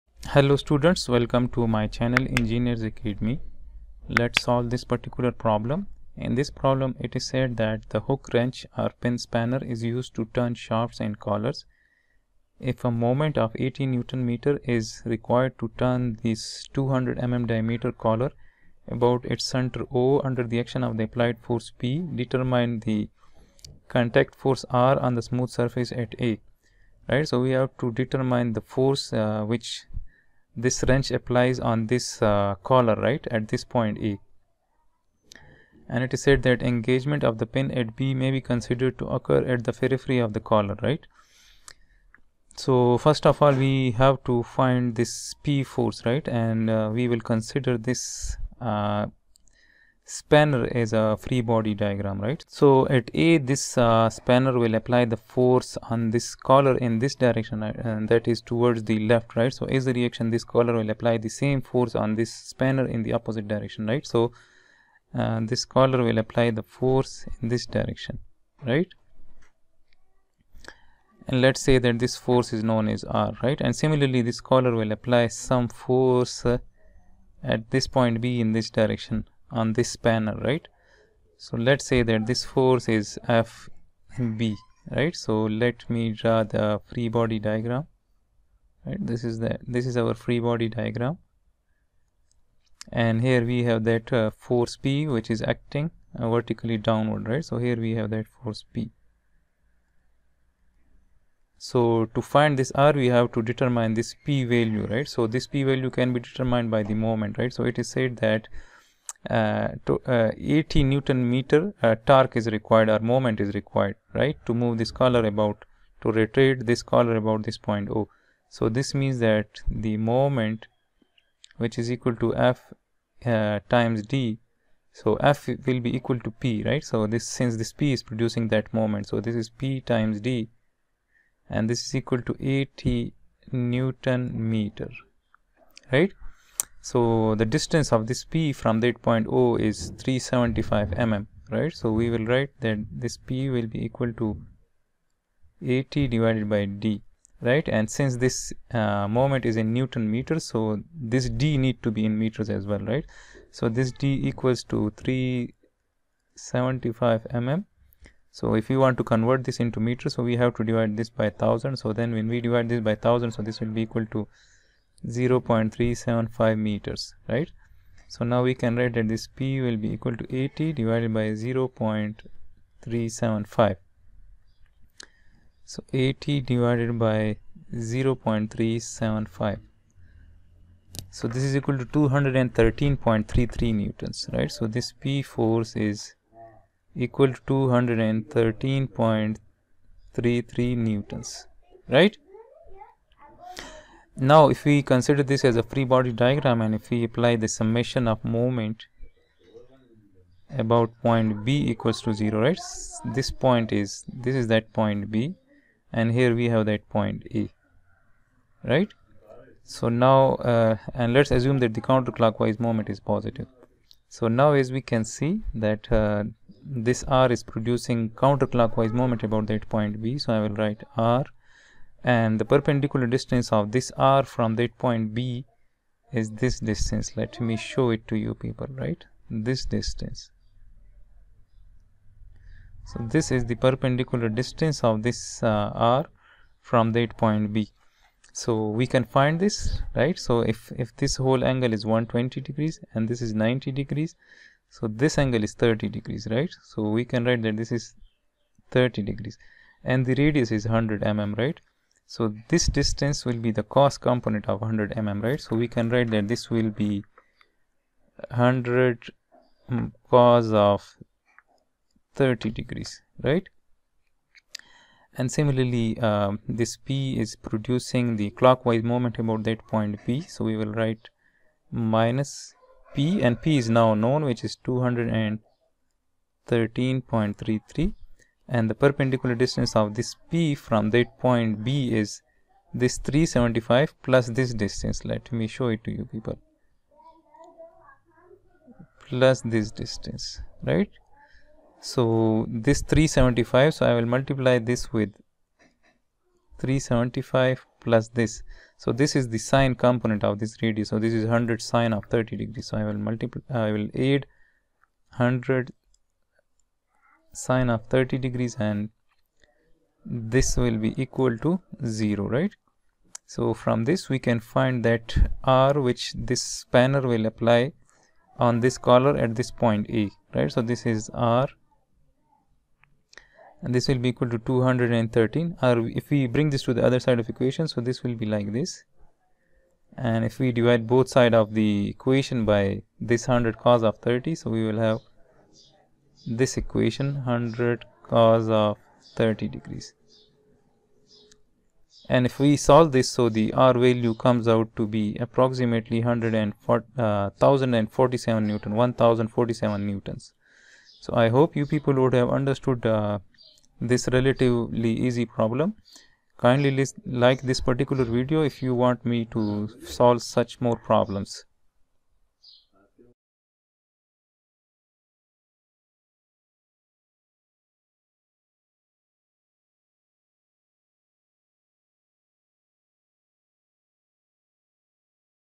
Determine the contact force R on the smooth surface. Chapter 3: Equilibrium | Engineers Academy
Chapter 3: Equilibrium

The hook wrench or pin spanner is used to turn shafts and collars. If a moment of is required to turn the 200-mm-diameter collar about its center O under the action of the applied force P, determine the contact force R on the smooth surface at A. Engagement of the pin at B may be considered to
occur at the periphery of the collar.